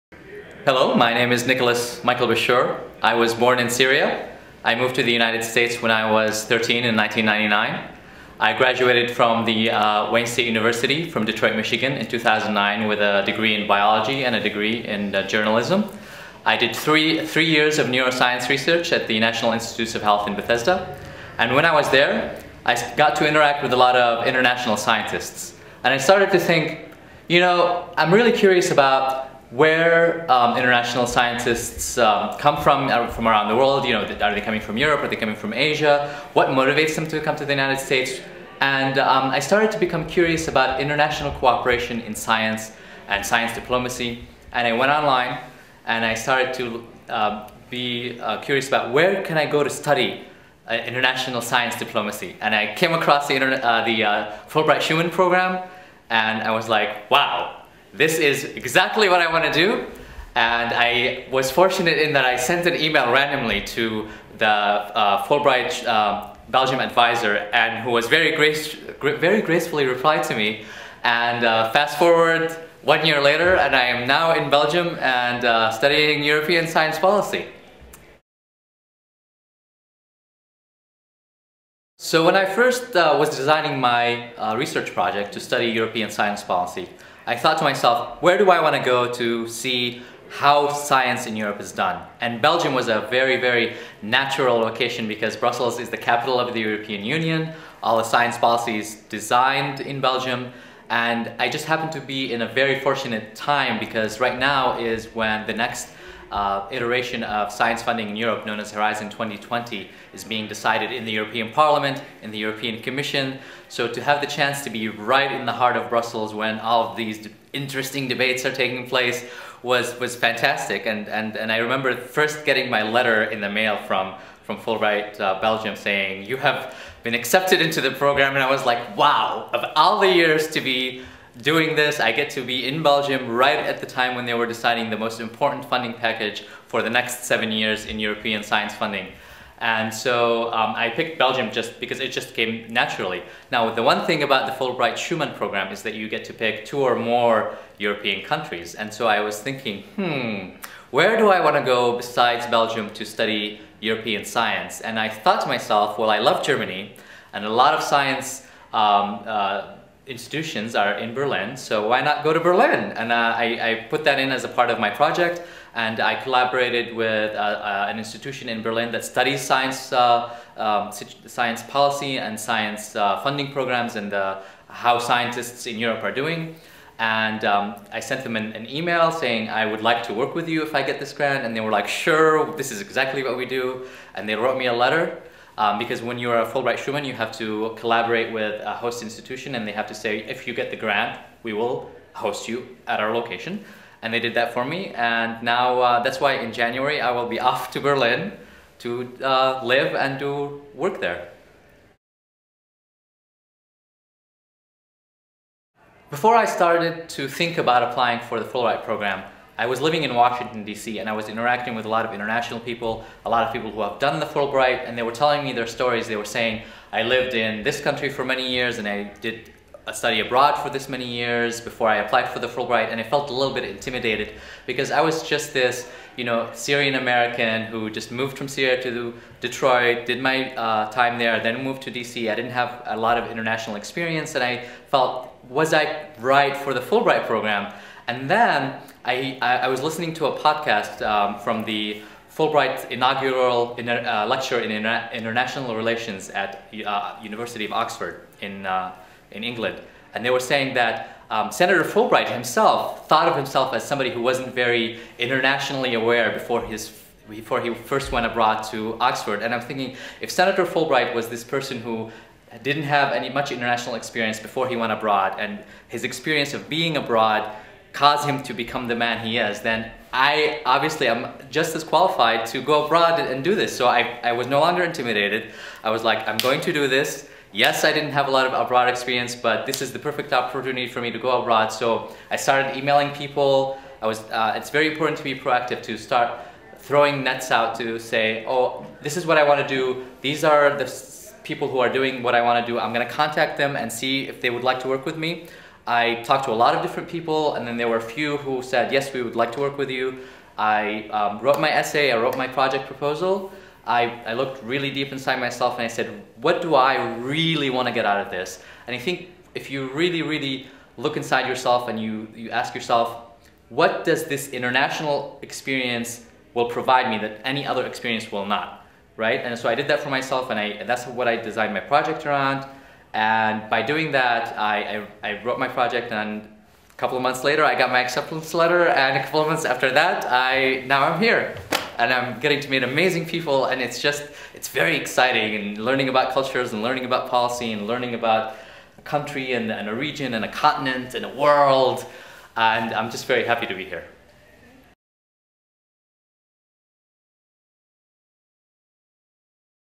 Nicholas Bashour | USEU Fulbright Schuman Grantee to Belgium, Germany
Nicholas Bashour was a Fulbright Schuman Grantee to the European Union. During his time as a participant in the Fulbright U.S. Student Program, he was based in Belgium and Germany. In this video, Nicholas talks about his research, his motivations for applying to the program, and his advice for future applicants.

Do you want to learn more about the US-EU Fulbright Schuman Program?